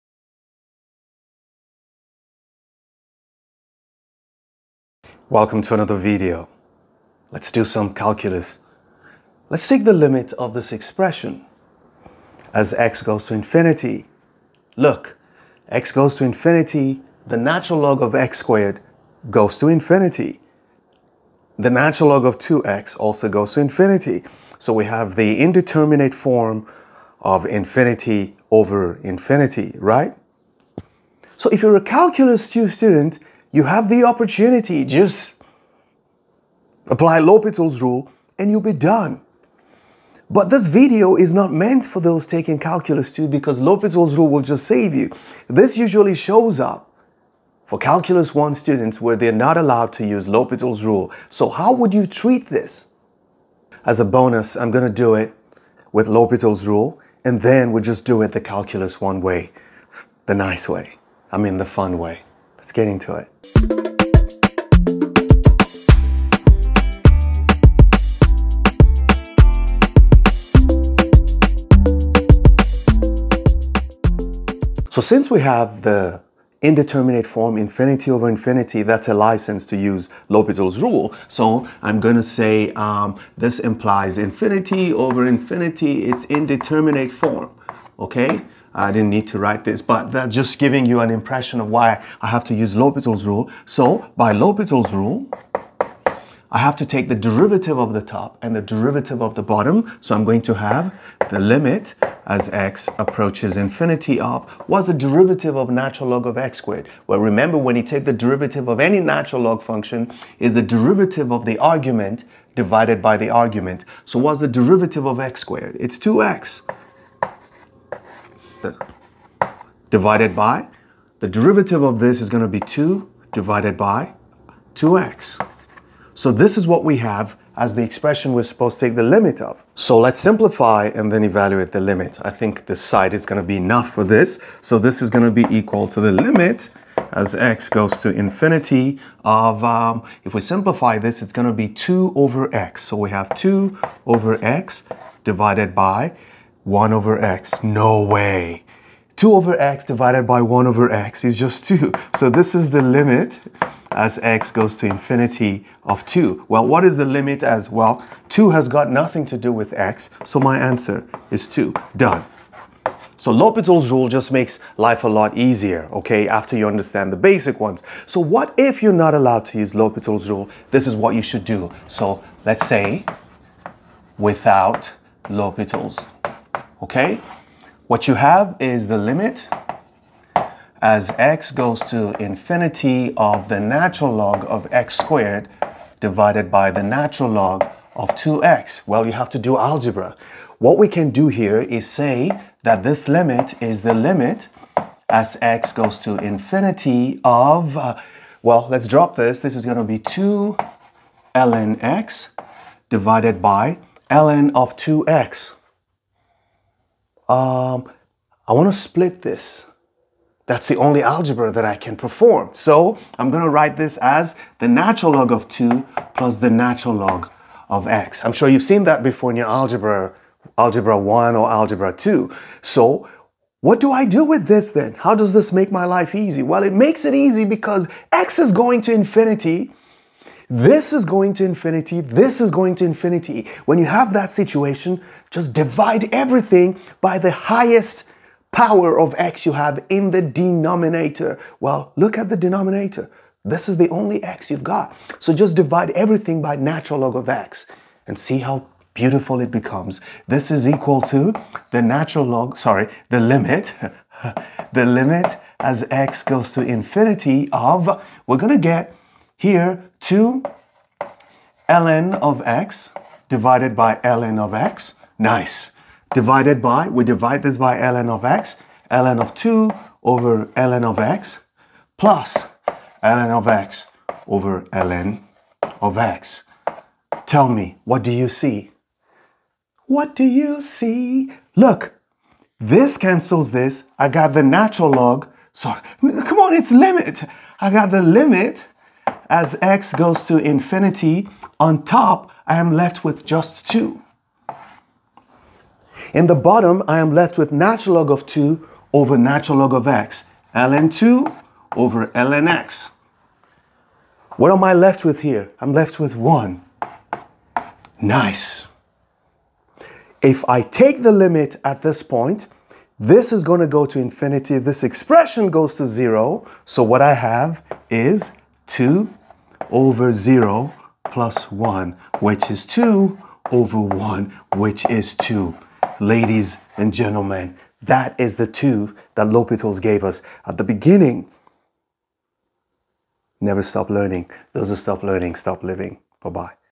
Limit of natural log @ infinity
In this video, I showed how tofind the limit of a rational function of logarithms as x goes to infinity